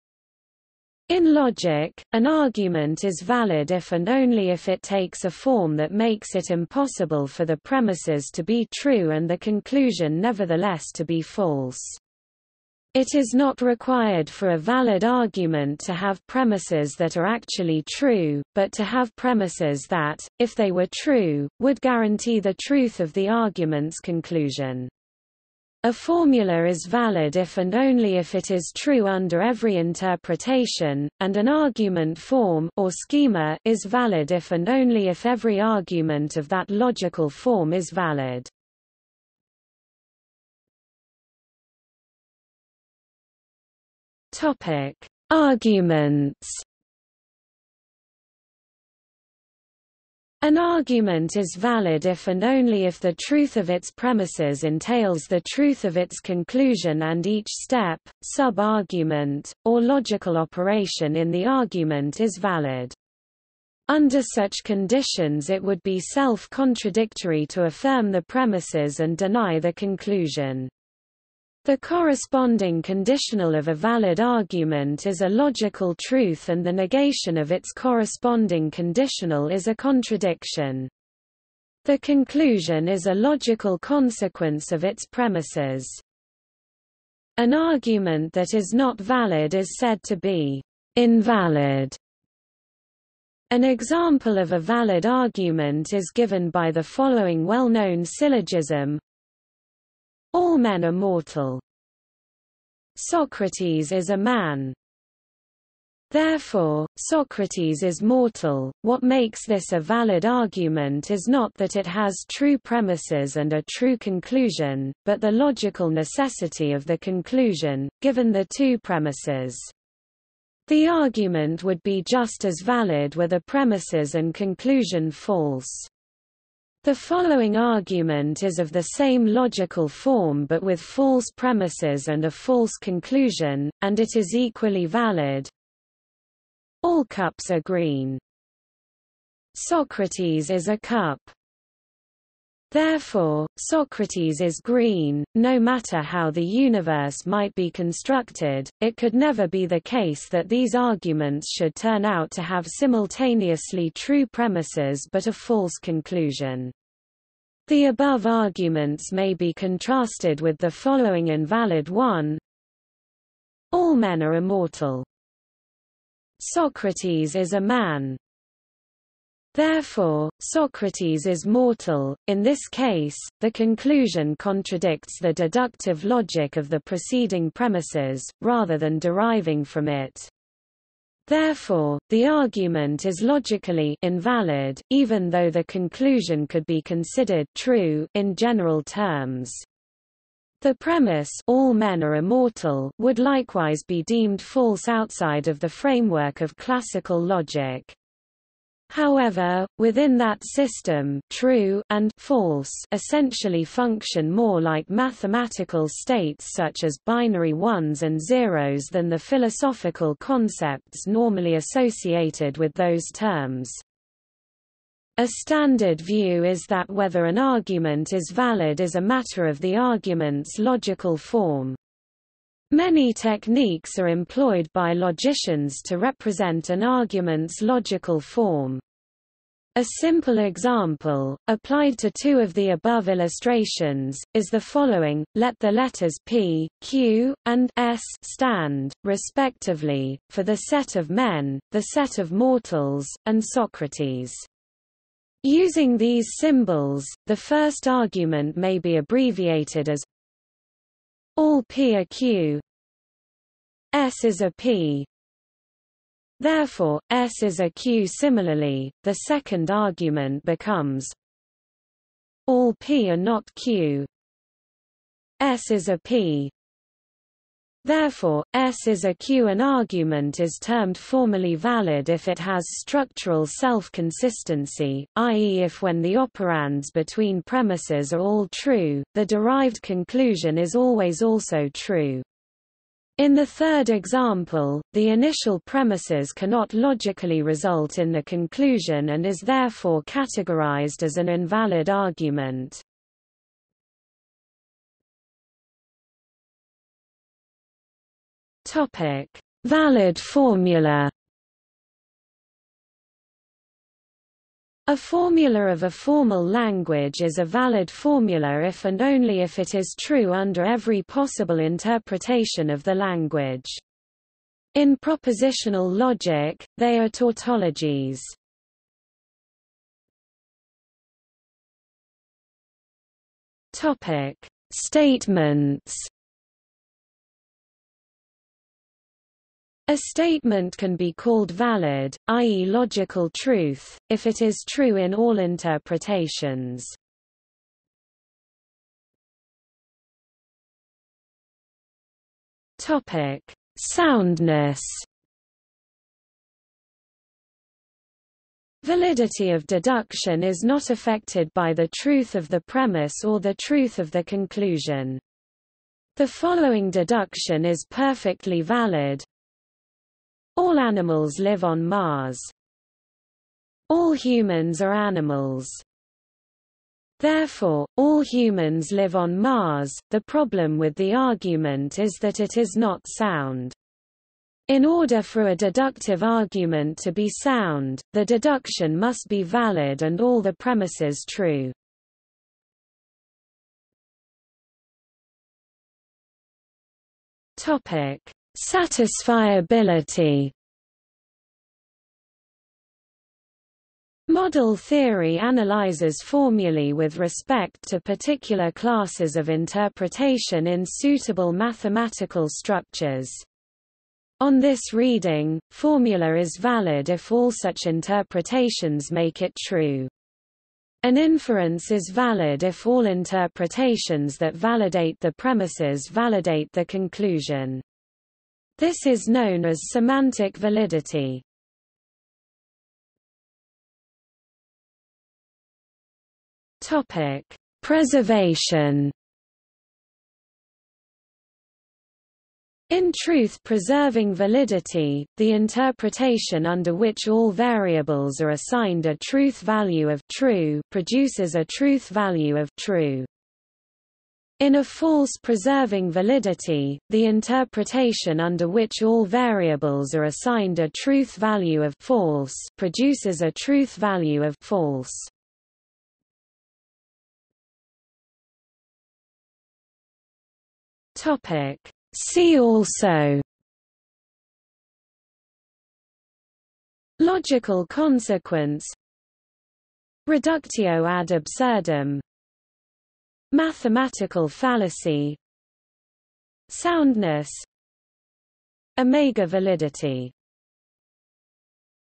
Validity (logic) | Wikipedia audio article
00:00:40 1 Arguments
00:04:55 2 Valid formula
00:05:20 3 Statements
00:05:39 4 Soundness
00:06:23 5 Satisfiability
00:07:03 6 Preservation
00:07:37 7 See also

- Socrates

SUMMARY
In logic, an argument is valid if and only if it takes a form that makes it impossible for the premises to be true and the conclusion nevertheless to be false. It is not required for a valid argument to have premises that are actually true, but to have premises that, if they were true, would guarantee the truth of the argument's conclusion. A formula is valid if and only if it is true under every interpretation, and an argument form (or schema) is valid if and only if every argument of that logical form is valid.